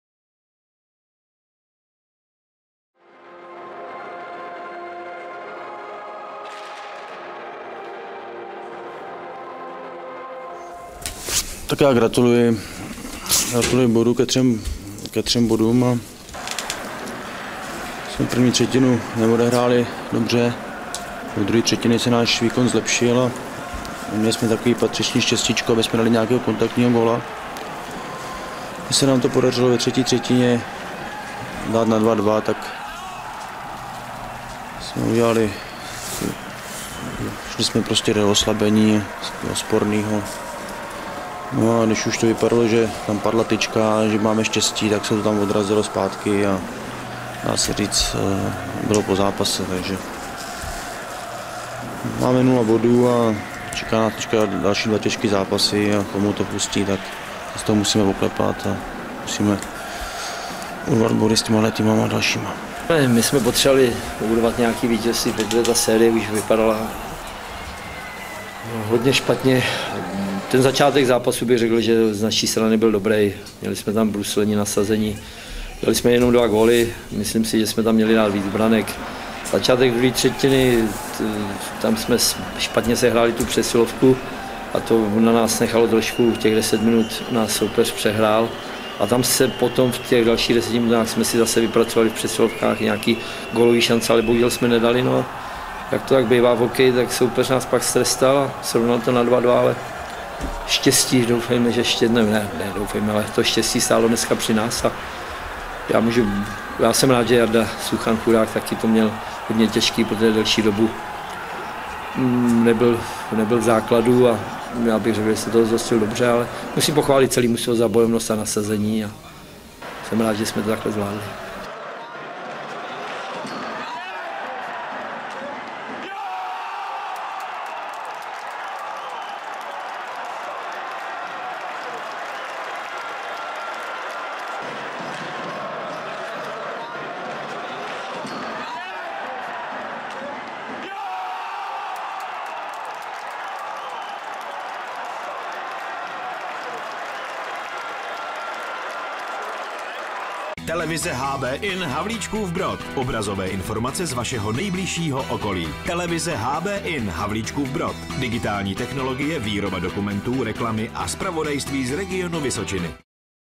HC REBEL HAVLÍČKŮV BROD - HC VCES HRADEC KRÁLOVÉ 3:2 (1 h.l. 2011/12)
HC REBEL HAVLÍČKŮV BROD - HC VCES HRADEC KRÁLOVÉ 3:2 (2:0, 0:0, 1:2)

Branky: 10. H. Zohorna, 11. Štěpánek, 56. Křivohlávek - 48. Berger, 54. Rufer.
Rozhodčí: Dvořák - Klein, Skopal.
Vyloučení: 7:7.
Využití: 2:1.
Diváci: 1109